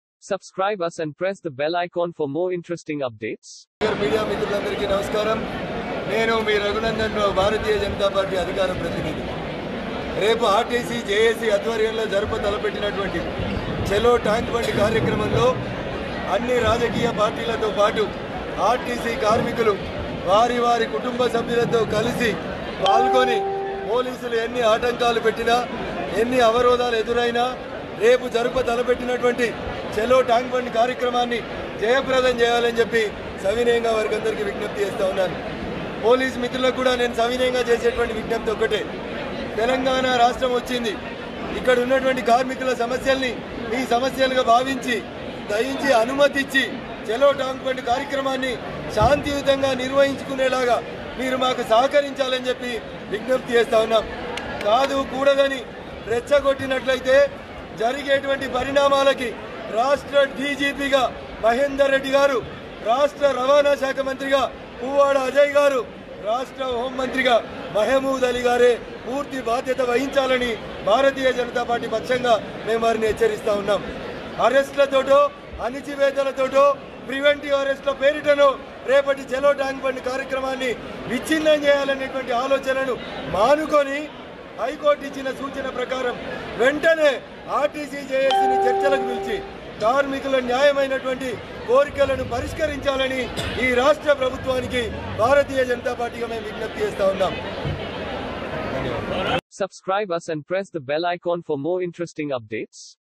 BJP Raghunandan Rao Serious Comments on KCR , Puvvada Ajay Kumar , DGP | BJP Telangana | TSRTC Stike
Raghunandan Rao Serious Comments on KCR , Puvvada Ajay Kumar , DGP | BJP Telangana | TSRTC Stike